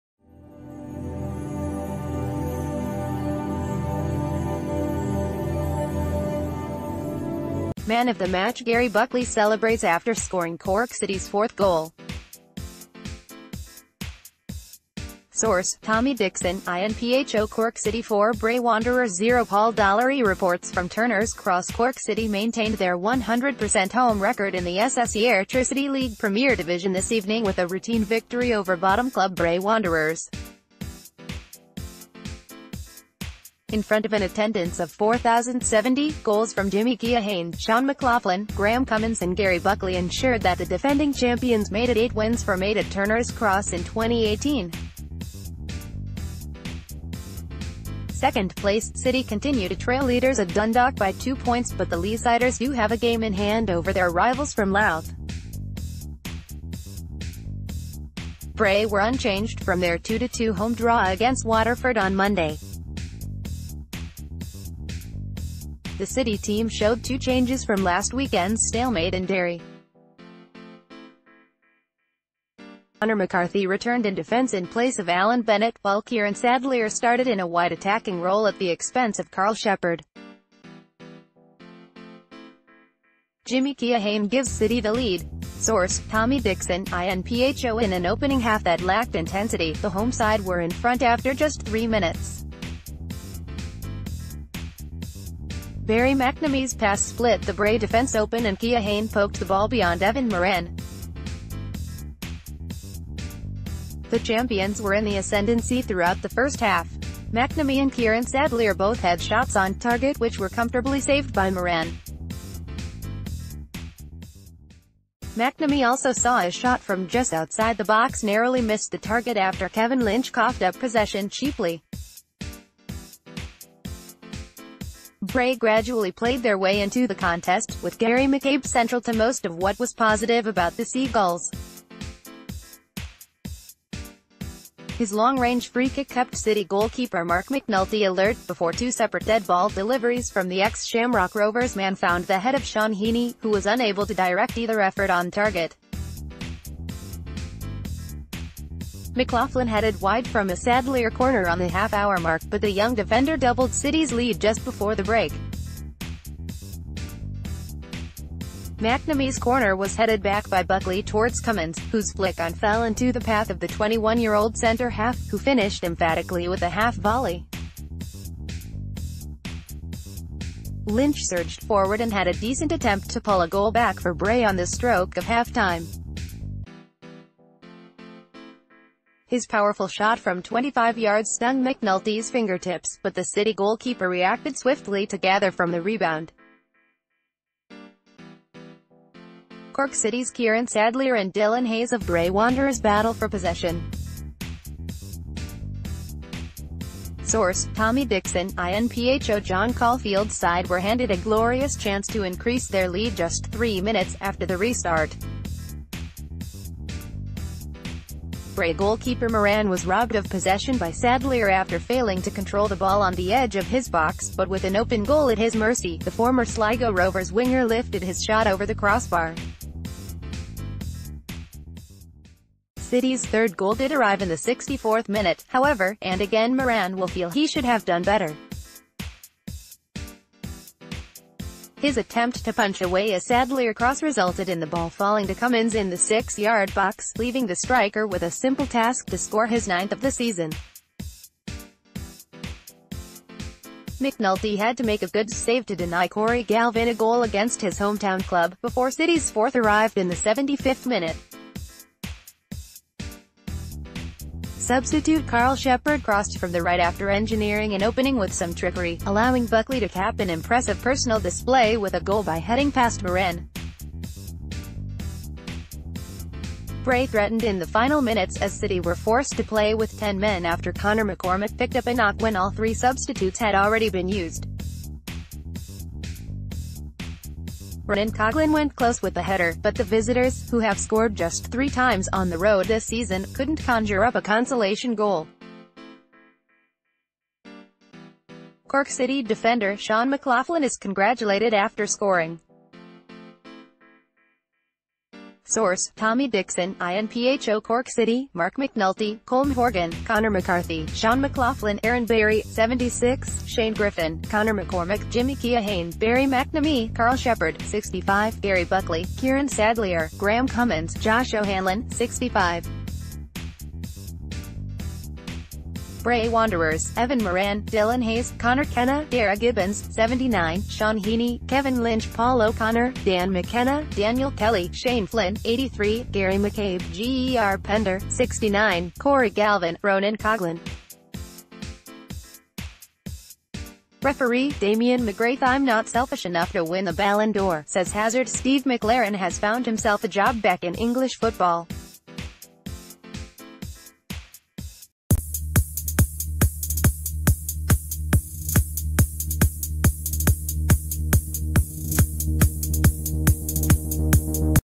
Breaking News | Four-goal Cork City keep perfect home record intact
Man of the match Garry Buckley celebrates after scoring Cork City's fourth goal. Source: Tommy Dickson/INPHO Cork City 4 Bray Wanderers 0 Paul Dollery reports from Turner’s Cross CORK CITY MAINTAINED their 100% home record in the SSE Airtricity League Premier Division this evening with a routine victory over bottom club Bray Wanderers. In front of an attendance of 4,070, goals from Jimmy Keohane, Sean McLoughlin, Graham Cummins and Garry Buckley ensured that the defending champions made it eight...